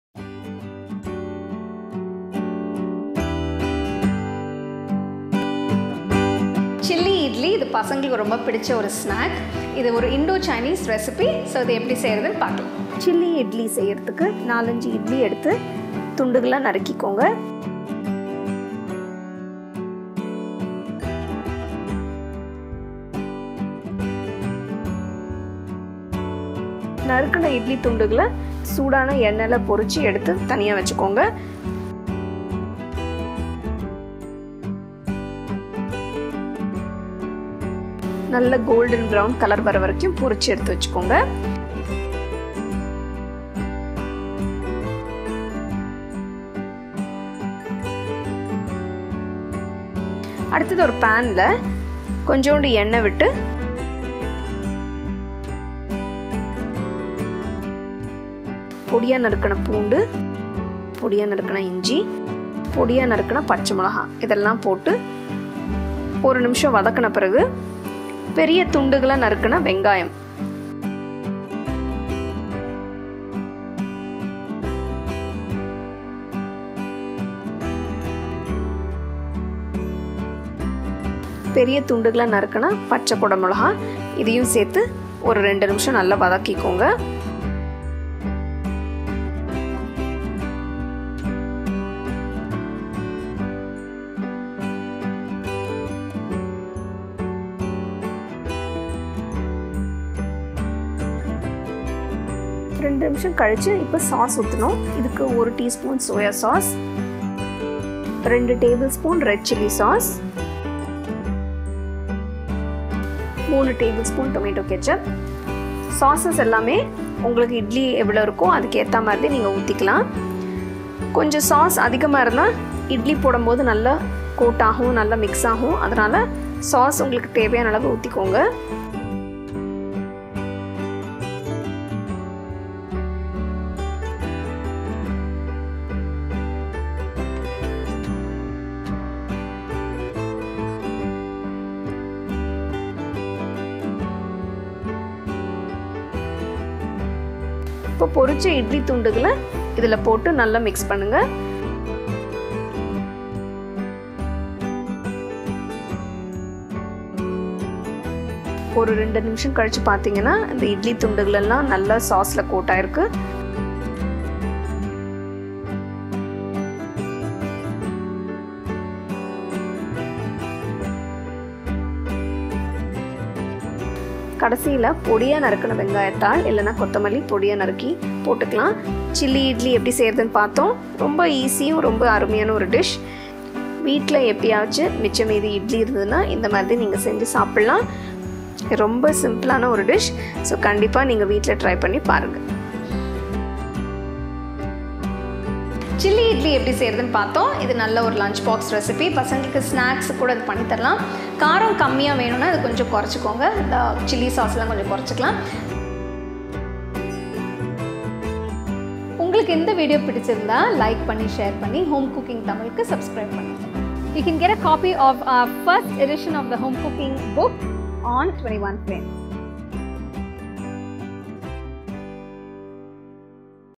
சில்லி இட்லி | chilli idli | Sunday Special|Cook Food|Samayal
சில்லி இட்லி  | chilli idli
தயாரிப்பு நேரம் - 15 நிமிடங்கள்
சமையல் நேரம் - 10 நிமிடங்கள்
தேவையான பொருட்கள்
இட்லி - 4
எண்ணெய்
பொடியாக நறுக்கிய பூண்டு
பொடியாக நறுக்கிய இஞ்சி
பொடியாக நறுக்கிய பச்சை மிளகாய்
நறுக்கிய பெரிய வெங்காயம் - 1
நறுக்கிய குடை மிளகாய் - 1/2
சோயா சாஸ் - 1 தேக்கரண்டி
ரெட் சில்லி சாஸ் - 2 மேசைக்கரண்டி
தக்காளி கெட்சப் - 3 மேசைக்கரண்டி
தேவையான அளவு உப்பு
தேவையான அளவு மிளகு தூள்
வெங்காயத்தாள் பச்சை பாகம் .
HAPPY COOKING WITH COOKFOOD
ENJOY OUR RECIPE